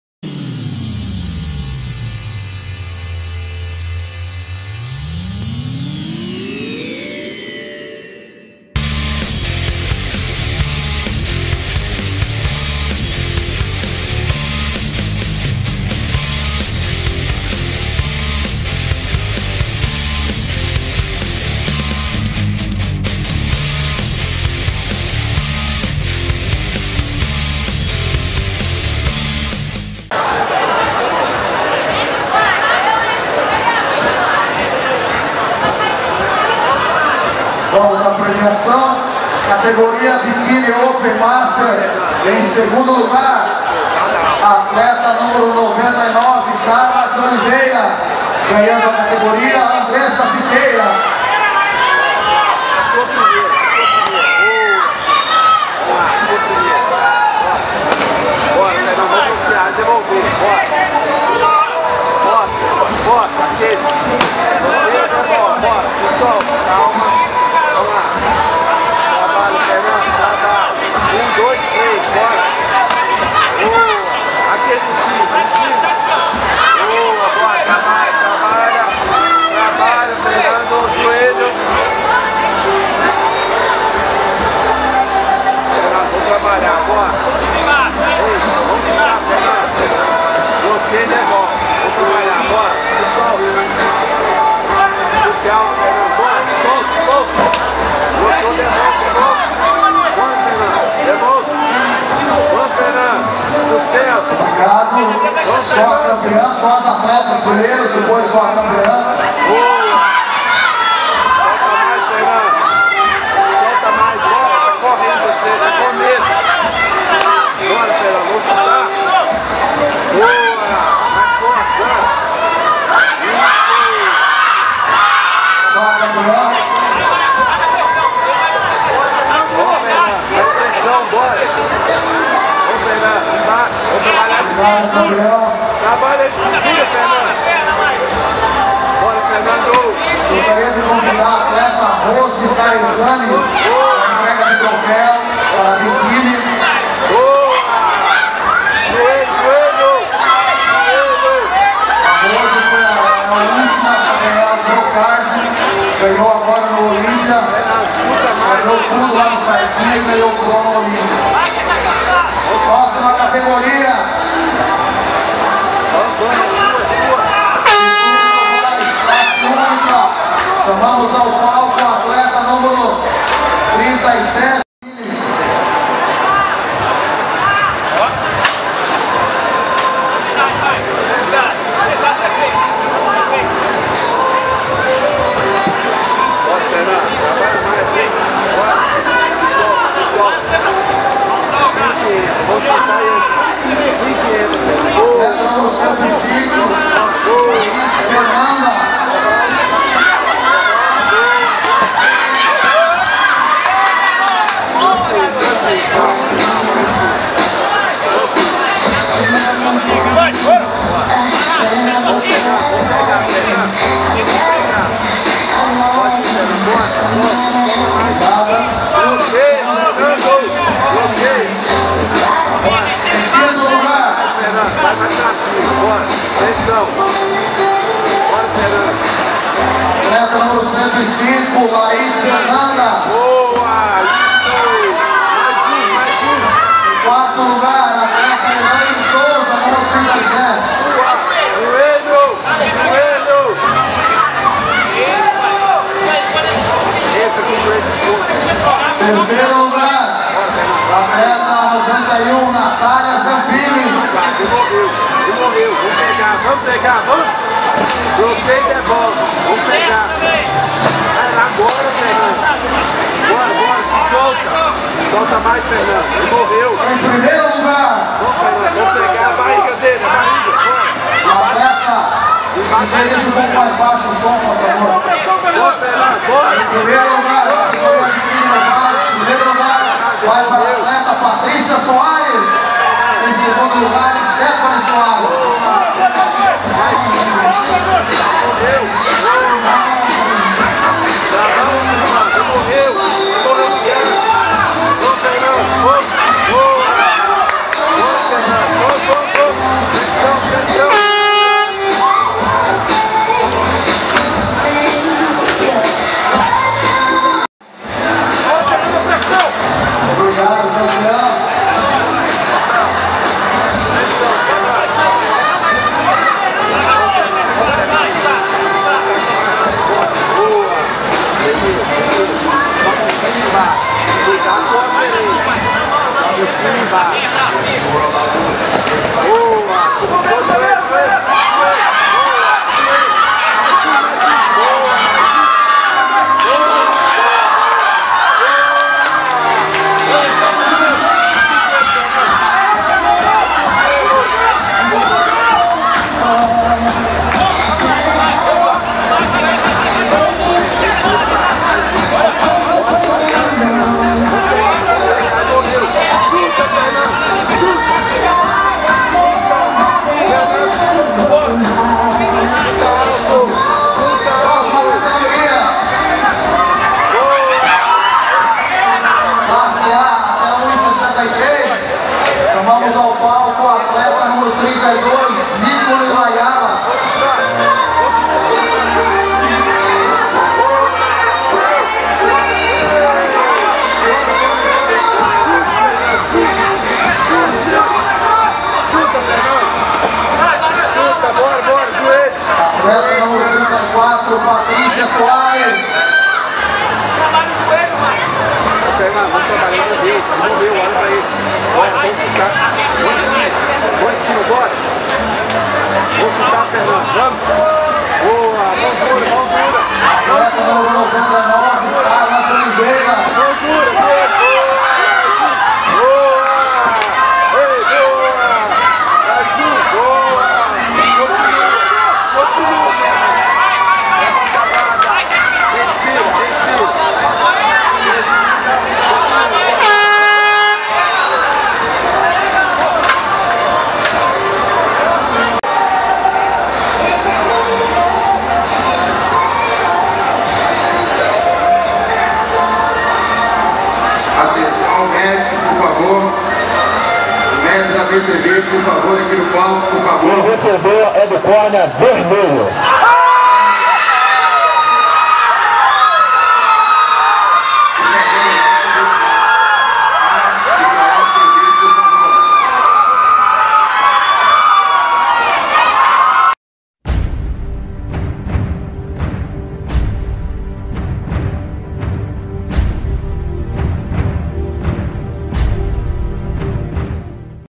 Fernando vs Maikon - Expo Fight Betim
Fernando Ferreira (Academia Elite) venceu Maikon Douglas (Tiger Lair Covil) por pontos